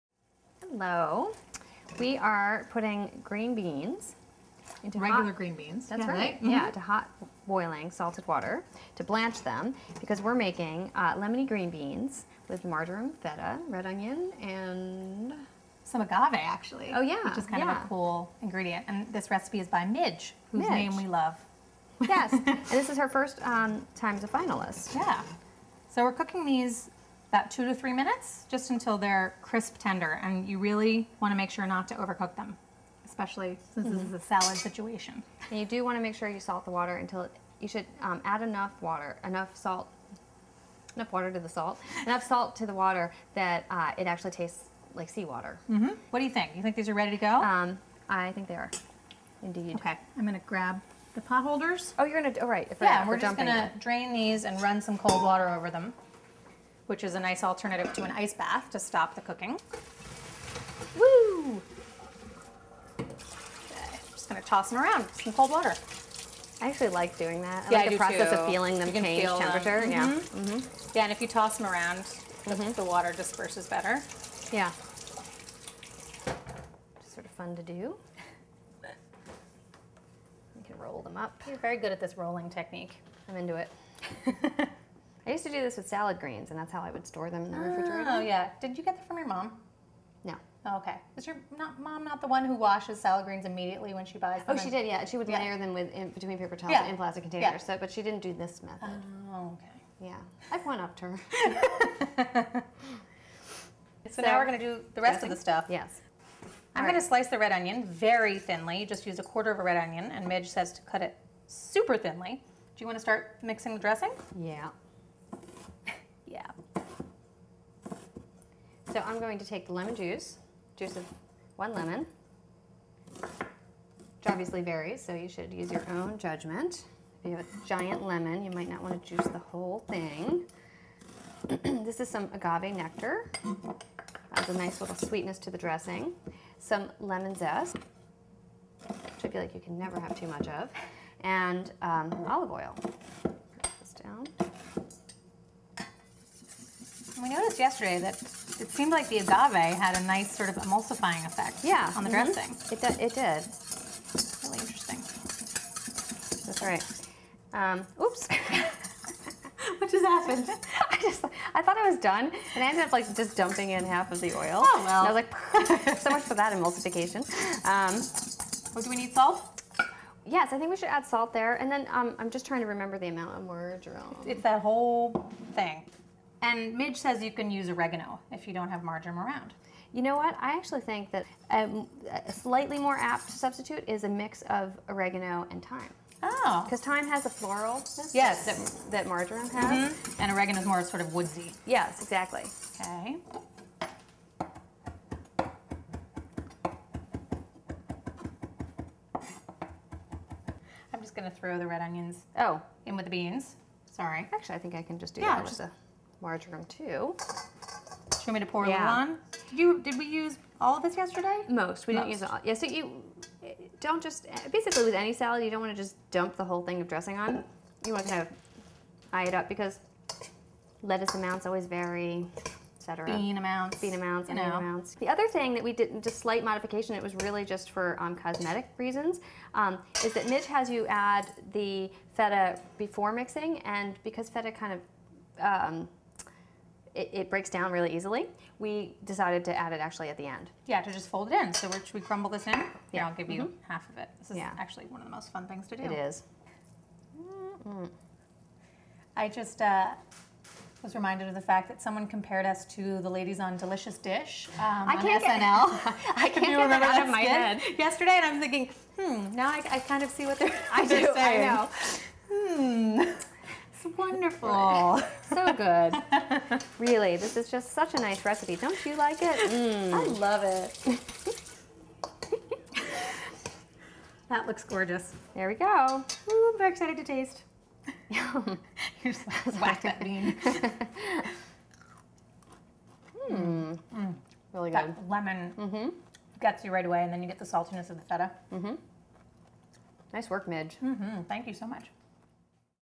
Your Best String Bean Salad: Lemony Green Bean Salad with Feta, Red Onion & Marjoram
Food52's Amanda and Merrill make Lemony Green Bean Salad with Feta, Red Onion & Marjoram. Full recipe below:

Have a Burning Food Question?